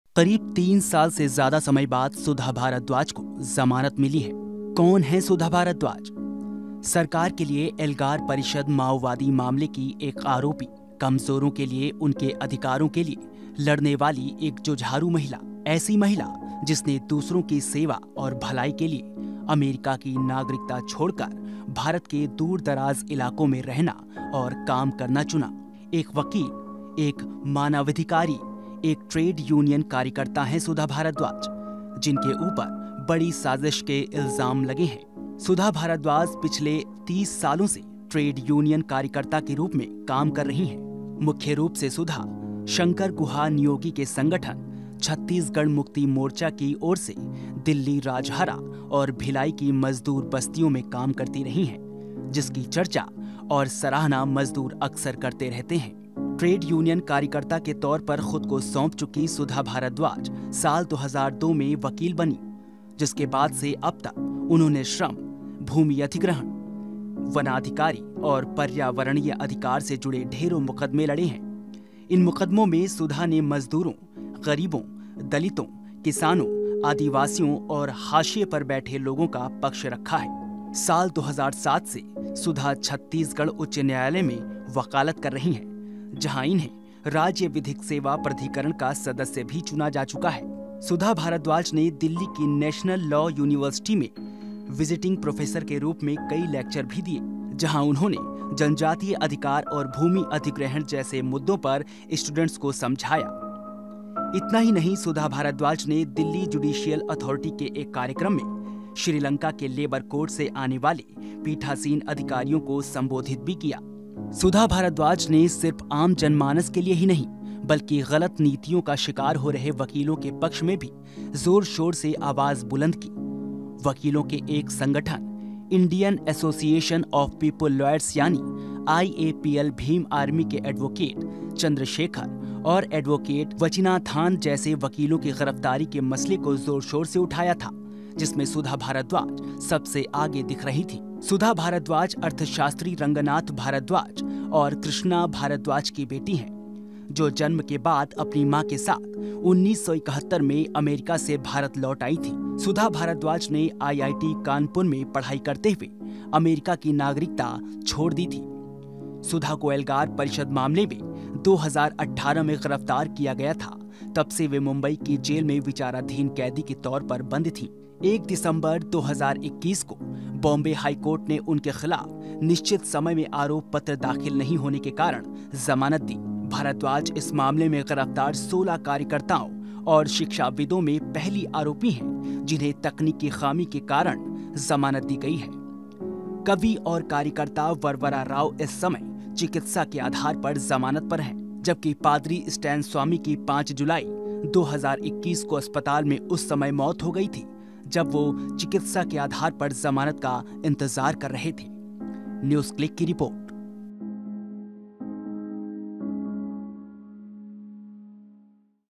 एल्गार परिषद मामले में सुधा भारद्वाज को ज़मानत मिली
बंबई उच्च न्यायालय ने एल्गार परिषद माओवादी संबंध मामले में वकील सुधा भारद्वाज को बुधवार को जमानत प्रदान कर दी। अदालत ने भारद्वाज को इस आधार पर जमानत प्रदान कि उनके खिलाफ निश्चित अवधि में आरोपपत्र दाखिल नहीं हुआ इसलिए वह जमानत की हकदार हैं।